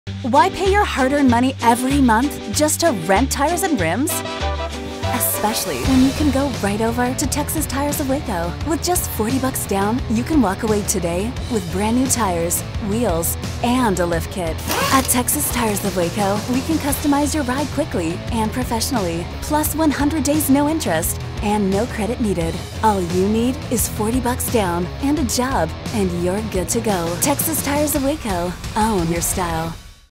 Texas Tires of Waco- $40 Down for Tires, Wheels, & Lift Kit
For just $40 down, you can drive away with New Tires, Wheels, & a Lift Kit for your truck, car, or SUV.